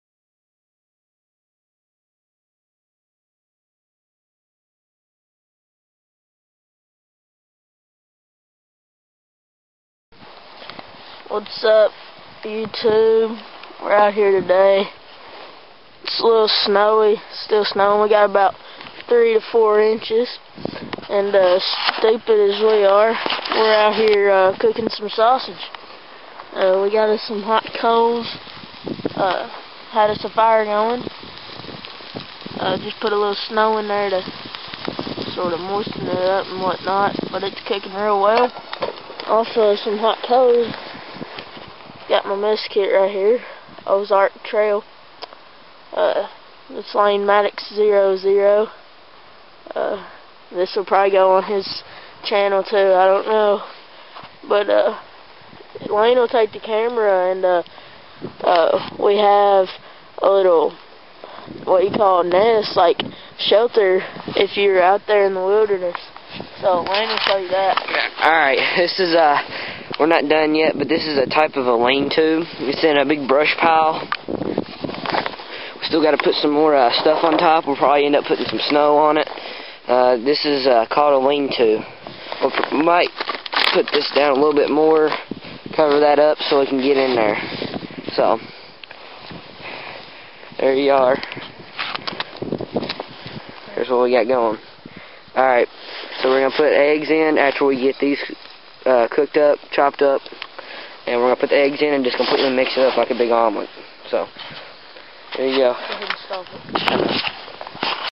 cookin sausage on hot coals
cooking sausage on a snowy day with mess kit and camp fire coals